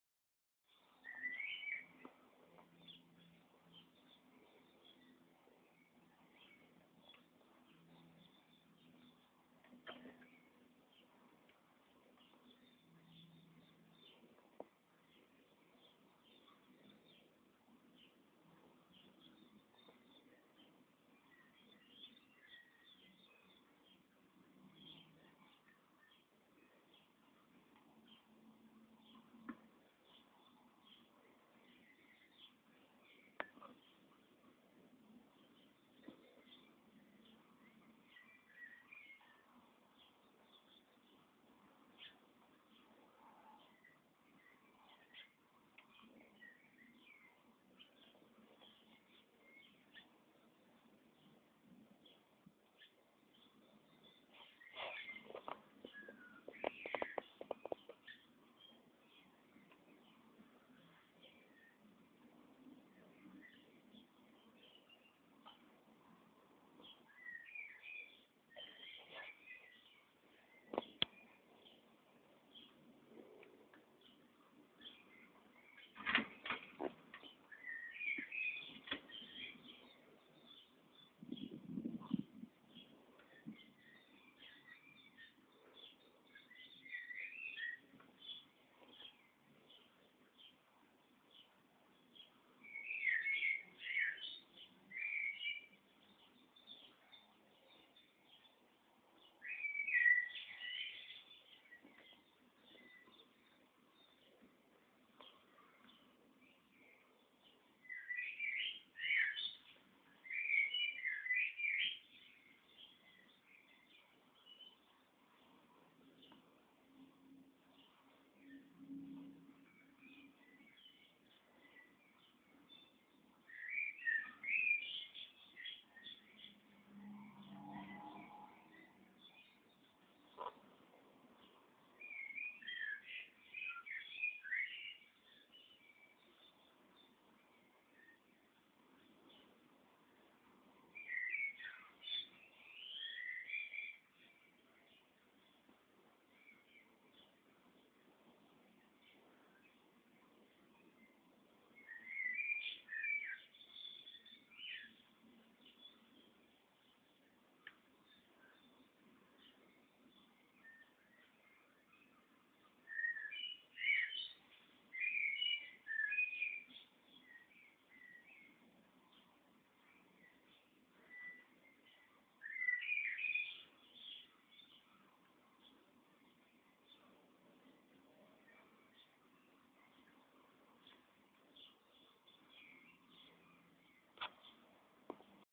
Jasper exploring the yard
Jasper moved into his new outside run yesterday and spent the night arranging his sleeping box to his liking. Today he's been out to enjoy the sunshine.